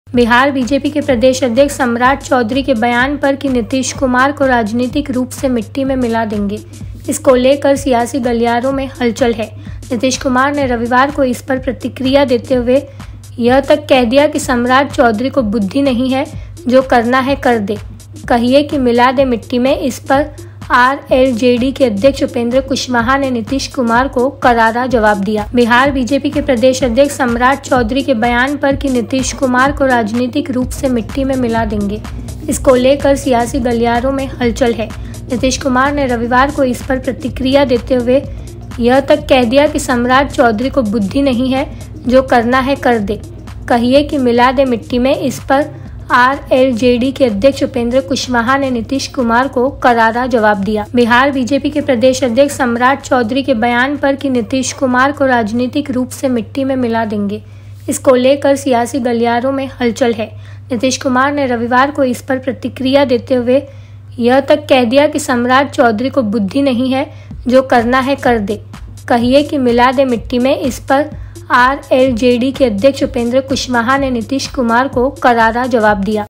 RLJD के अध्यक्ष Upendra Kushwaha ने नीतीश कुमार को करारा जवाब दिया
बिहार बीजेपी के प्रदेश अध्यक्ष सम्राट चौधरी (Samrat Choudhary) के बयान पर कि नीतीश कुमार (Nitish Kumar) को राजनीतिक रूप से मिट्टी में मिला देंगे. इसको लेकर सियासी गलियारे में हलचल है. नीतीश कुमार ने रविवार (23 अप्रैल) को इस पर प्रतिक्रिया देते हुए यहां तक कह दिया कि सम्राट चौधरी को बुद्धि नहीं है. जो करना है कर दे. कहिए कि मिला दे मिट्टी में. इस पर आरएलजेडी के अध्यक्ष उपेंद्र कुशवाहा (Upendra Kushwaha) ने नीतीश कुमार को करारा जवाब दिया है.उपेंद्र कुशवाहा ने रविवार (23 अप्रैल) को मीडिया से बातचीत में कहा कि नीतीश कुमार तो खुद ही अपने बारे में बोलते रहते हैं. कुशवाहा ने कहा कि जिस शब्द (मिट्टी) का इस्तेमाल सम्राट चौधरी ने किया है वह स्वयं नीतीश कुमार अपने बारे में बोलते रहे हैं कि मिट्टी में मिल जाएंगे लेकिन यह काम नहीं करेंगे. राजनीति की डिक्शनरी में इस शब्द का इस्तेमाल तो नीतीश कुमार ने ही किया है. आज अगर उनके बारे में कहीं से इस्तेमाल हो रहा है तो नीतीश कुमार को सोचना चाहिए कि हमने जब इस्तेमाल किया था खुद अपने बारे में तो उन्हें करना चाहिए था कि नहीं.